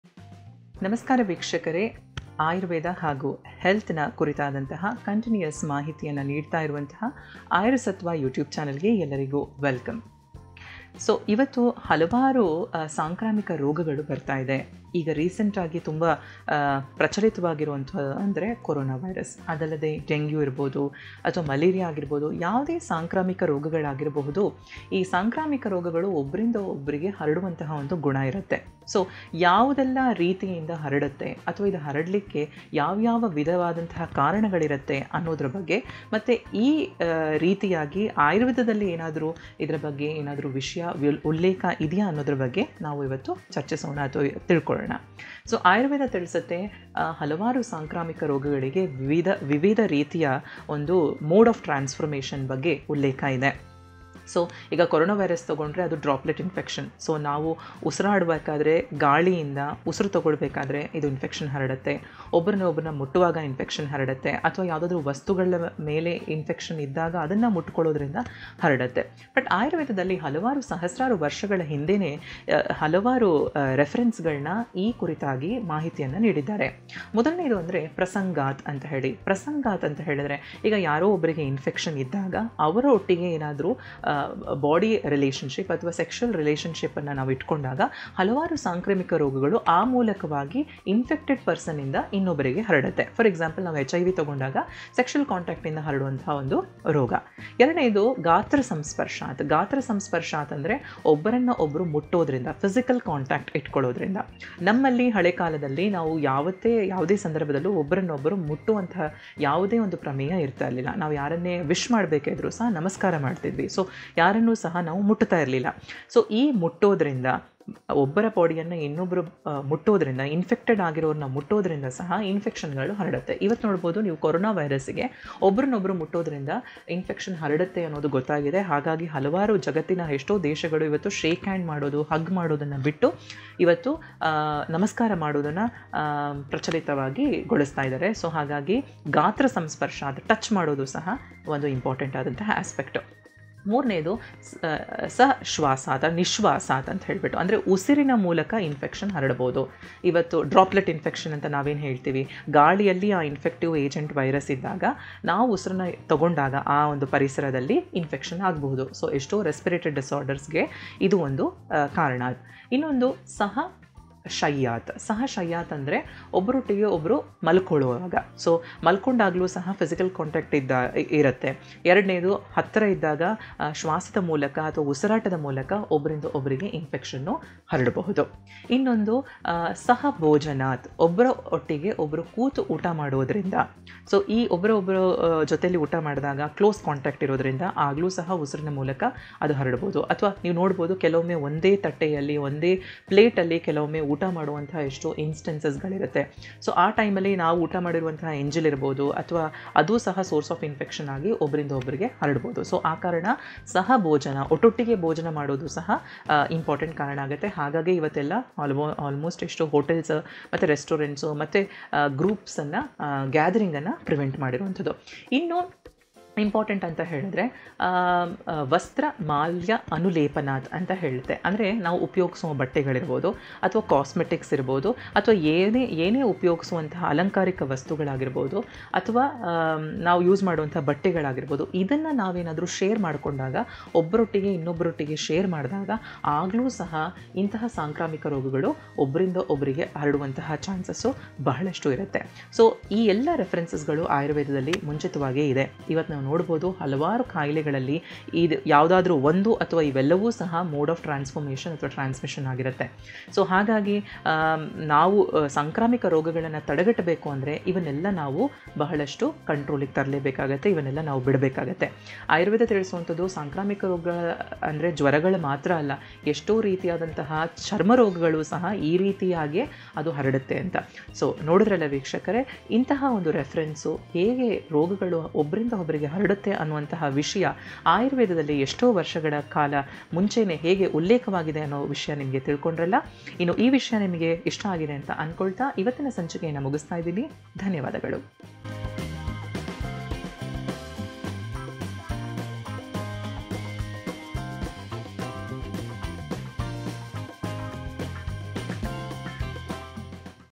ಸಾಂಕ್ರಾಮಿಕ ರೋಗ (Infectious Diseases) ಹರಡುವ ವಿಧಾನಗಳು..!
ಸಾಂಕ್ರಾಮಿಕವಾಗಿ ರೋಗಗಳು ಯಾವೆಲ್ಲ ರೀತಿಯಲ್ಲಿ ಹರಡಬಹುದು ಎಂಬುದಕ್ಕೆ ಆಯುರ್ವೇದದಲ್ಲಿ ಉಲ್ಲೇಖಿತವಾಗಿರುವ ಅಂಶಗಳನ್ನು ಈ ವೀಡಿಯೋದಲ್ಲಿ ಹೇಳಲಾಗಿದೆ.